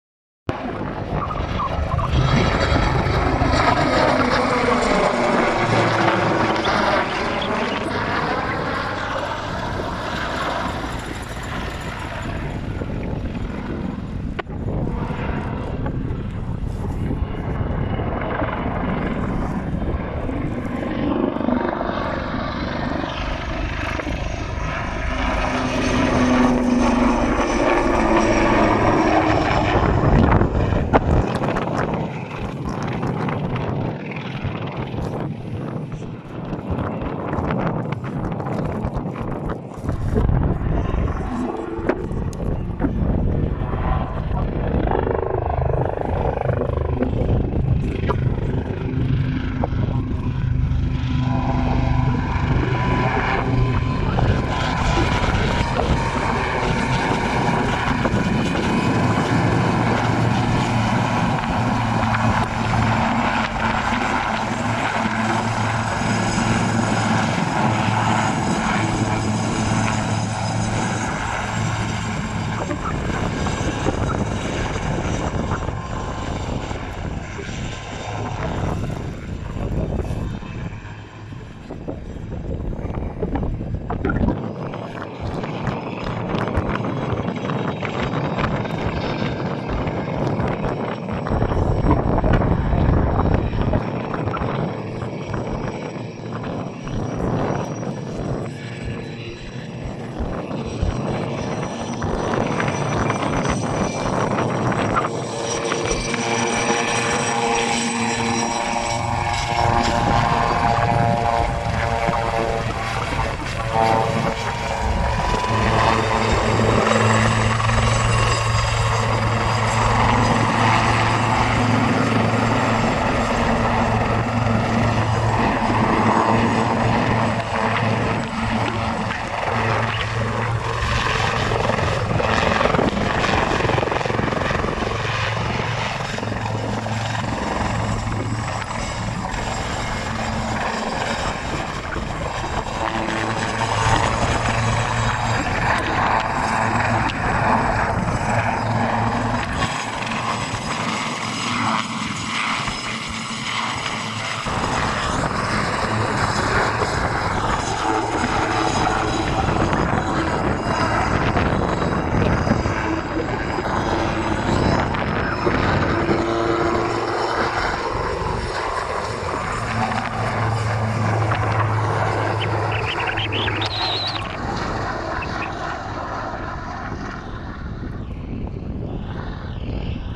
ACTION MIT HUBSCHRAUBER SEA LYNX beim Fest Himmel und Erde Flugplatz Bienenfarm
Am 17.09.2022 zeigt der Marine Bord Hubschrauber SEA LYNX beim Fest am Flugplatz Bienenfarm wenige Kilometer westlich von Berlin was er und seine Besatzung so alles drauf hat. Hier ein paar technische Daten des Hubschraubers. Rumpf Länge 13,30 m, Breite 2,90 m, Höhe 3,70 m, Rotordurchmesser 12,80 m, maximales Startgewicht 5,3 t, Leistung 2.200 PS, Höchstgeschwindigkeit 325 km/h, Verbrauch 380 Liter Kerosin je Stunde, Reichweite 530 km in ca. 2:20 h.